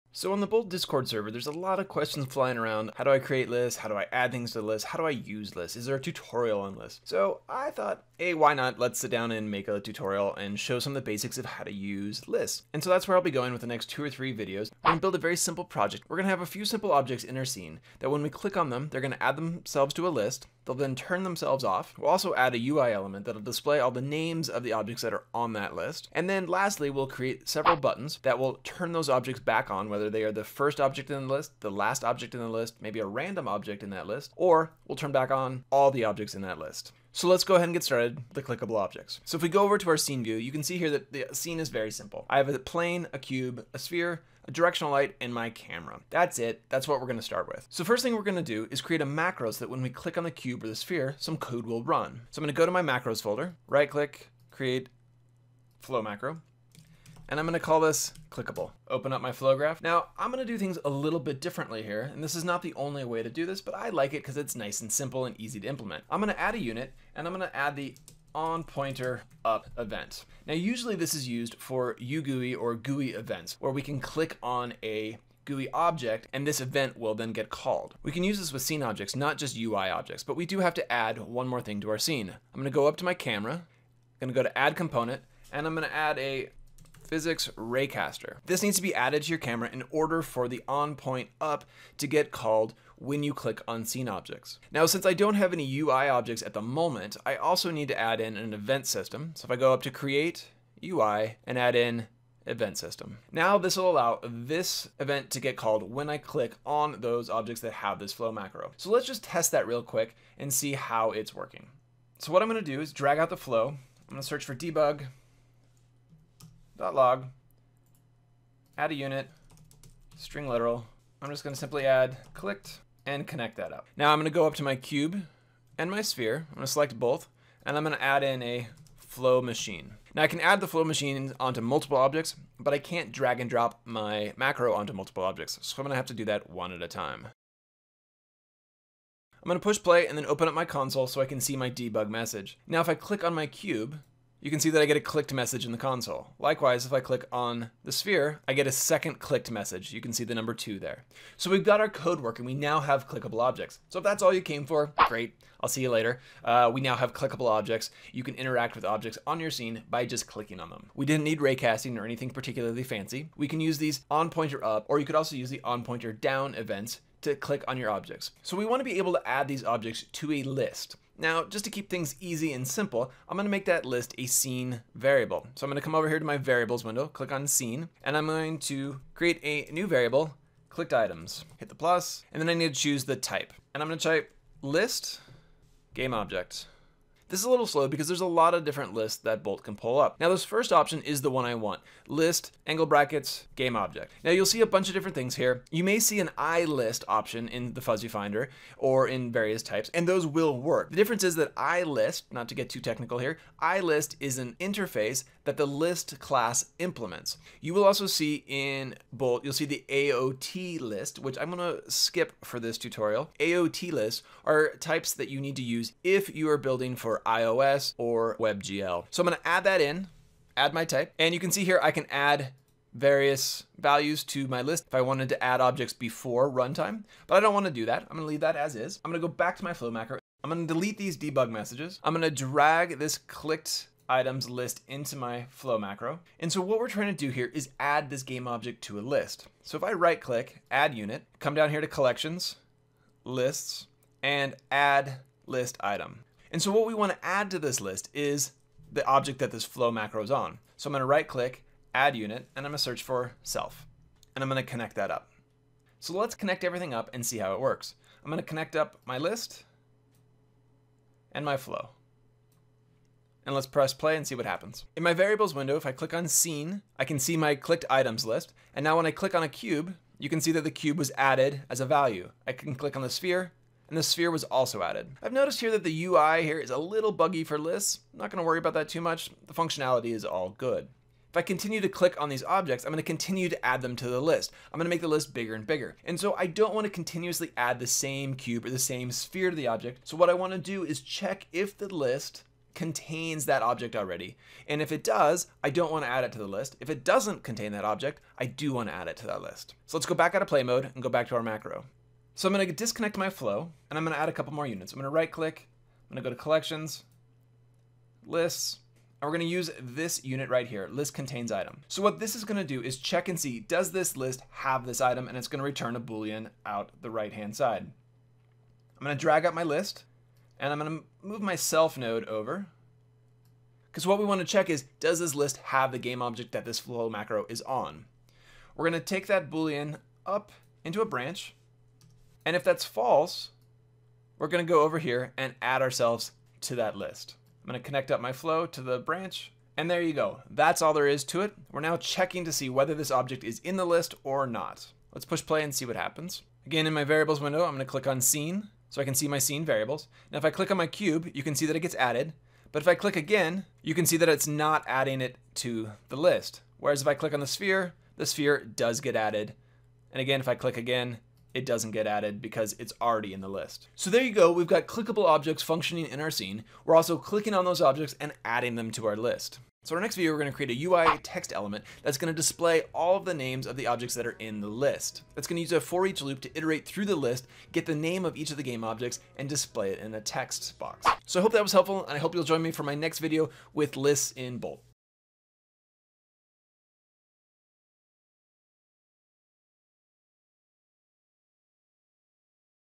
Using Lists with Bolt Visual Scripting
A Bolt tutorial on Lists! This video looks at the basics of adding objects to a list and does so by creating "clickable scene objects" that when clicked add themselves to a list. All using Bolt visual scripting.